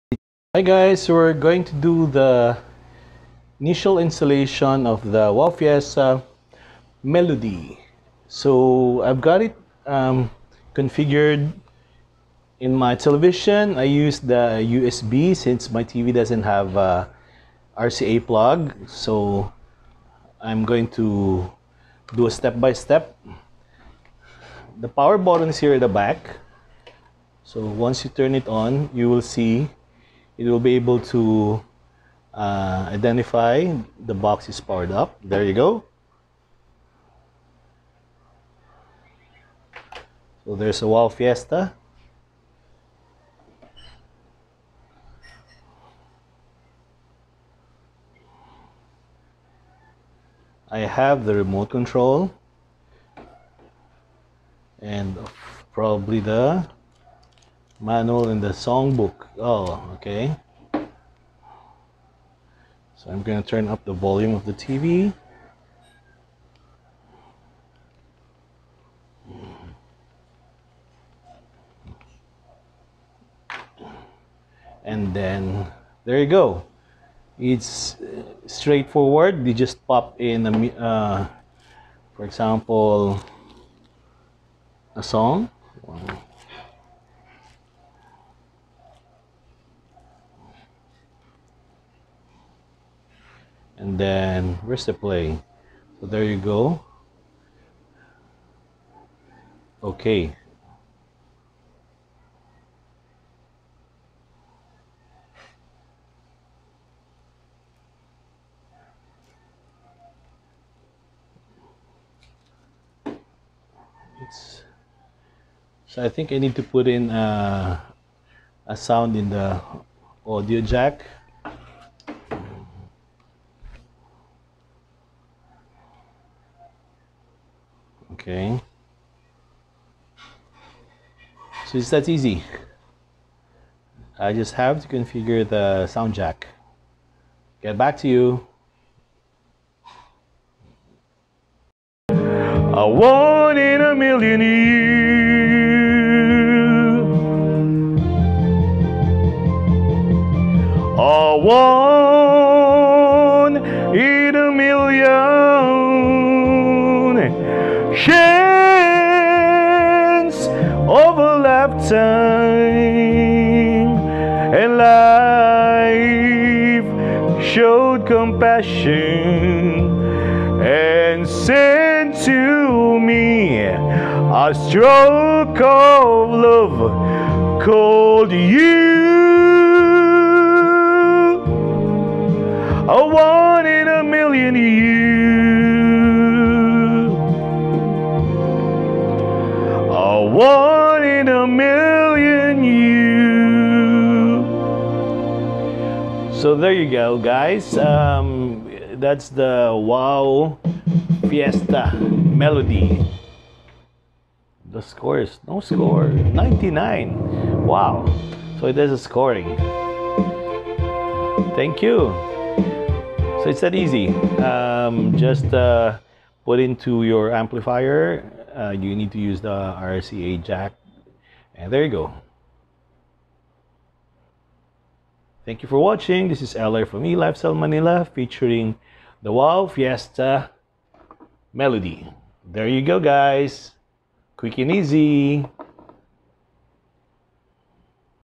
How to configure Wow! FIESTA Melody
Wow Fiesta Melody is one of the latest entry model that you can buy and enjoy with the whole family